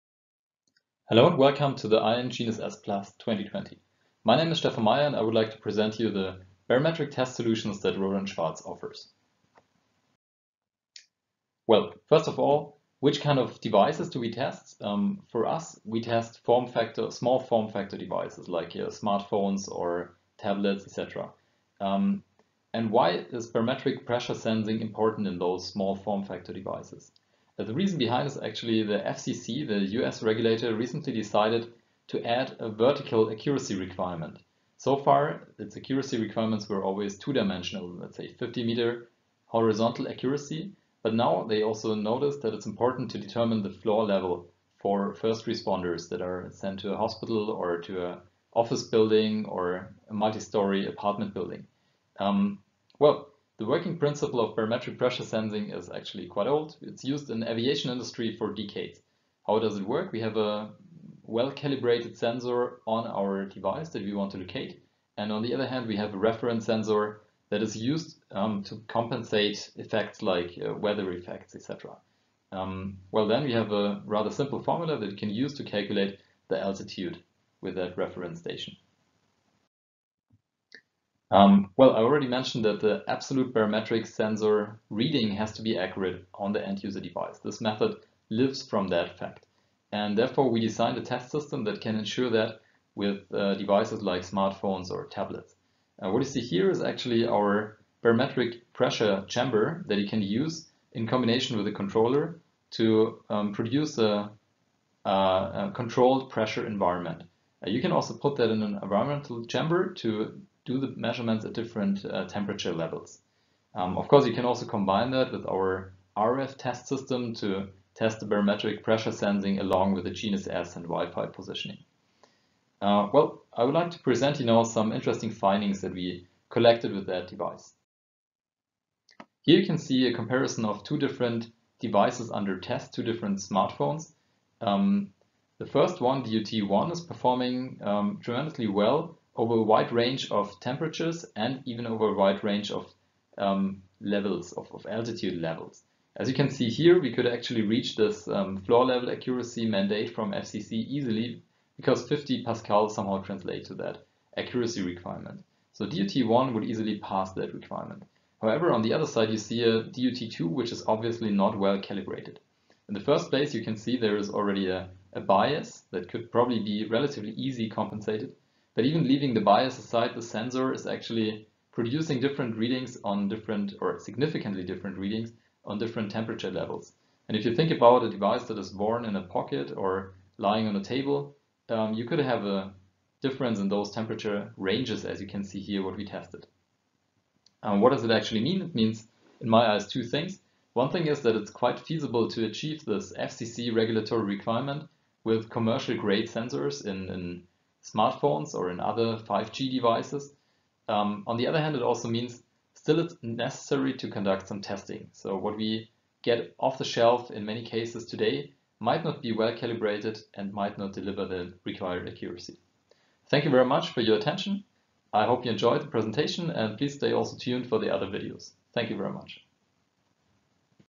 Testing to the new vertical accuracy requirements of the FCC Wireless E911
To meet the FCC indoor Wireless E911 Location Accuracy Requirements, barometric pressure mandate of the FCC, vertical (z-axis) accuracy must meet a +/- 3m accuracy. This video  describes the FCC requirement, test methodology, and need for performing barometric pressure device calibration.

More information about the FCC Wireless E911 Location Accuracy Requirements (page 8)